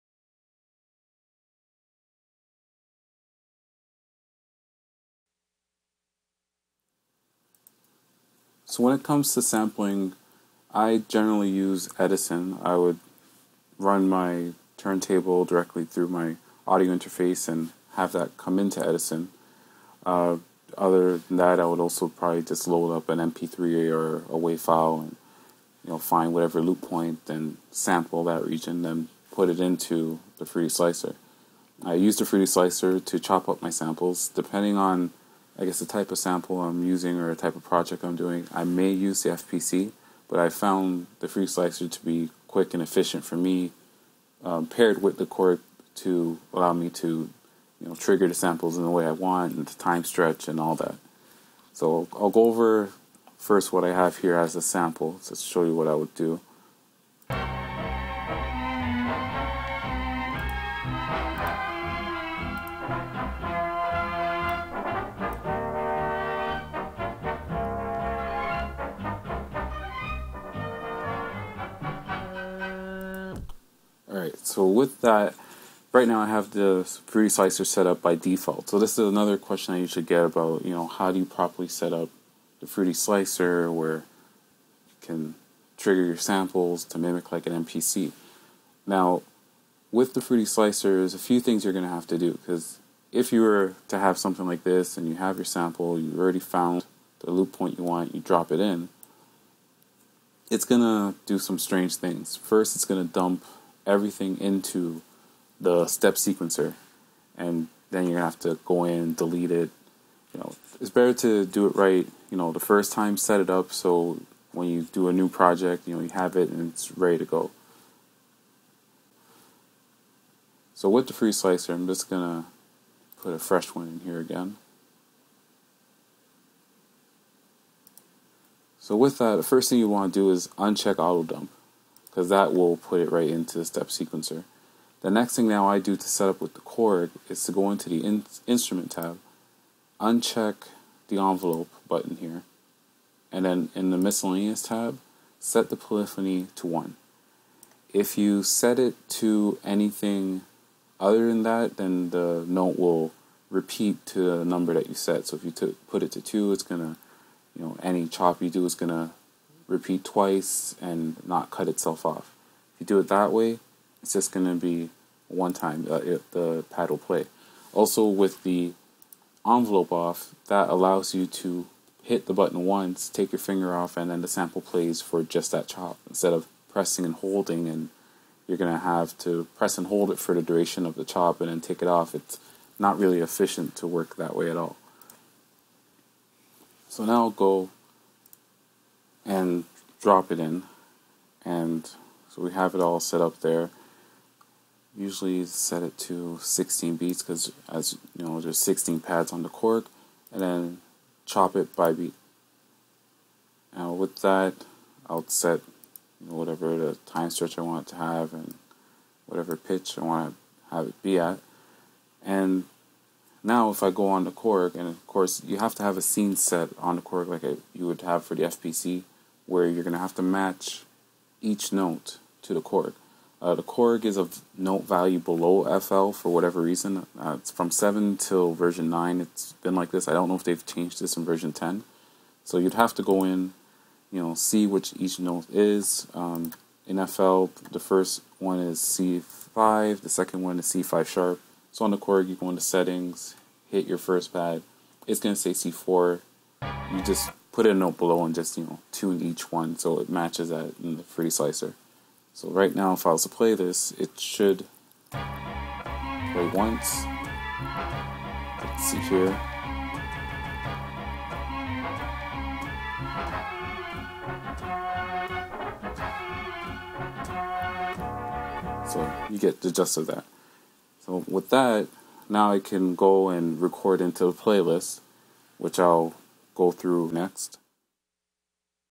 FL Padkontrol Tutorial Part 3 - (FLSlicer)
I try to go over and answer some commonly asked questions about the Padkontrol and FL.

The series of videos briefly overviews how I do the following:
- Setting up the Korg within options
- Triggering Samples in the FPC and FLSlicer

Hopefully there is some valuable information in the vid for everyone.

Note: Some of the overlays didn't show up (thanks to my capture program), but everything should be self explanatory if you have used FL Studio before.